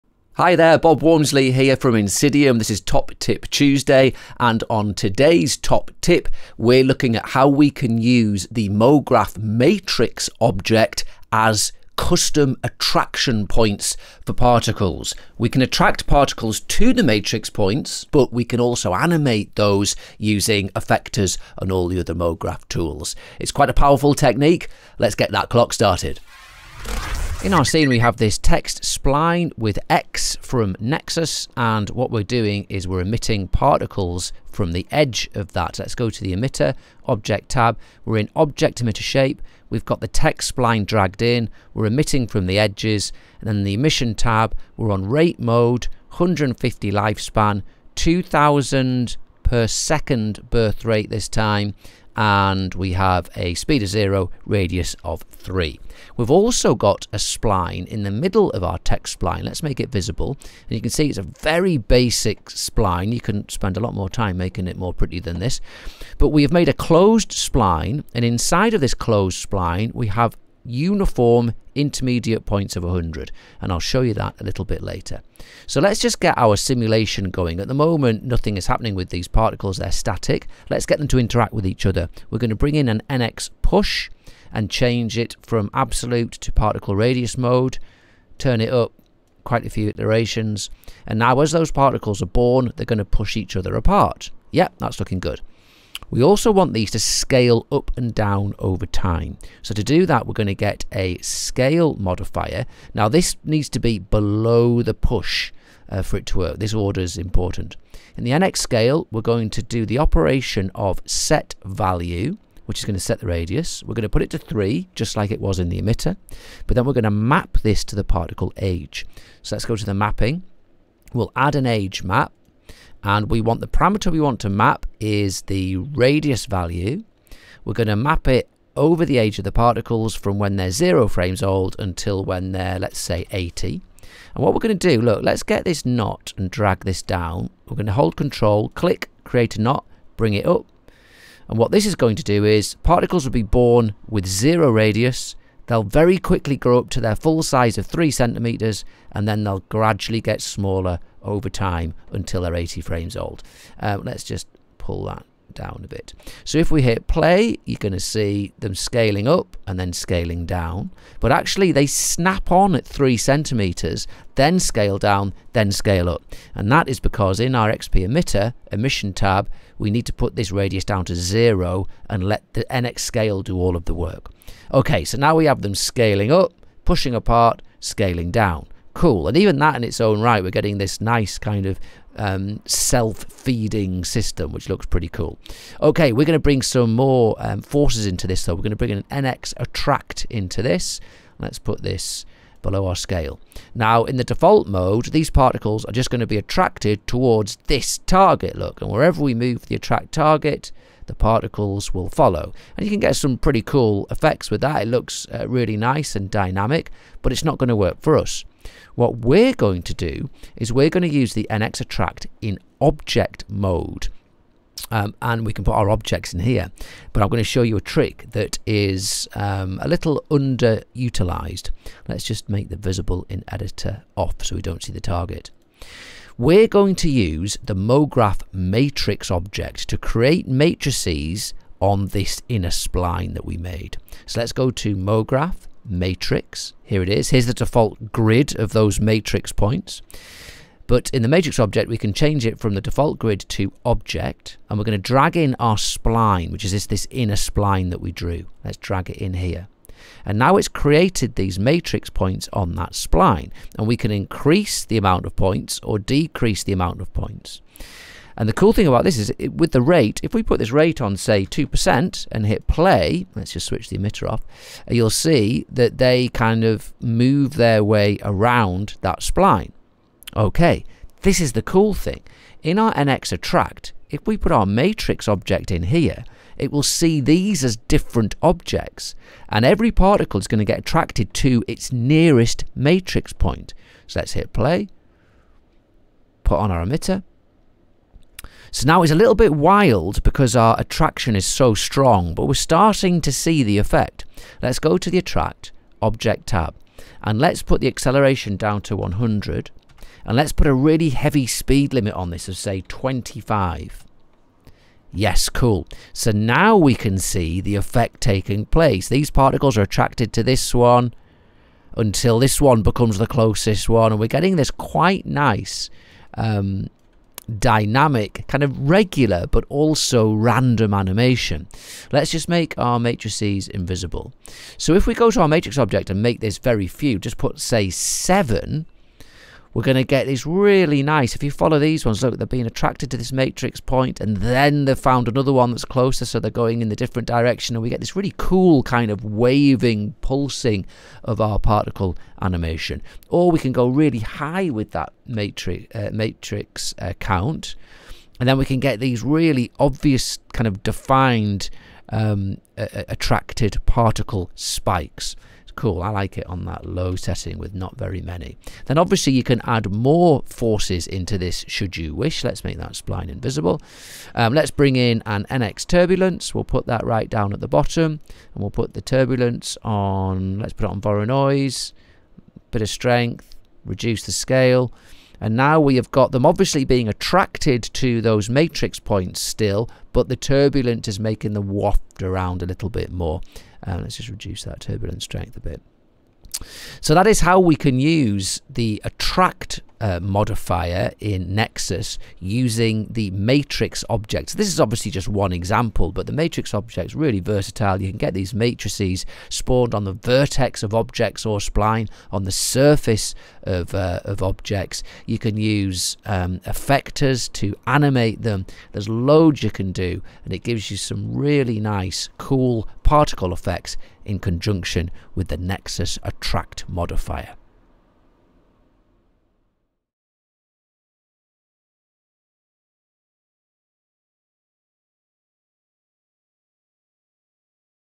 Top Tip Tuesday - Matrix Particle Attract
In this week’s Top Tip Tuesday, particles are generated from a spline object and attracted to the center of the scene using an nxAttract modifier. Instead of using the object axis center as a single attraction point, multiple points are generated using the Matrix object. Particle-to-particle interaction is then added using nxPush, creating a dynamic flow to the particle movement.

Have you checked out our Content Repository? FREE scenes to download & learn with!